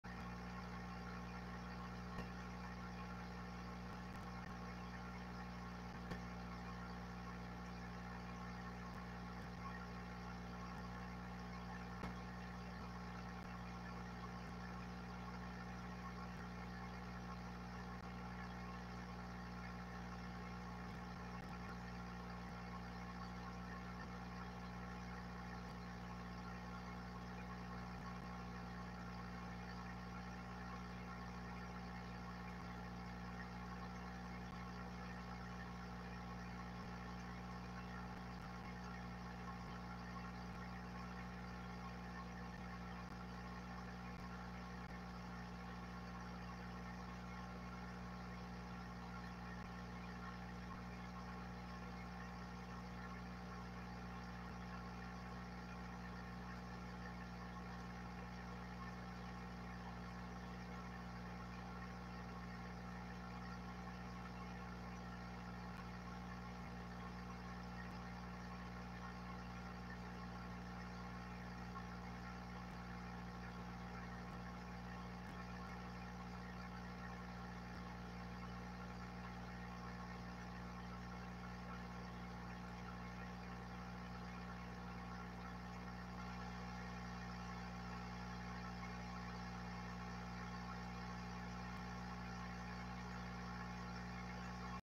Fish Tank ASMR - Bubbling sounds 🐠 🐟
Relax and fall asleep watching these beautiful creatures, and listening to the oxygen bubbler. Tetras, mollies, guppies, catfish and algae eater. And of course our star Ulysses the friendly beta fish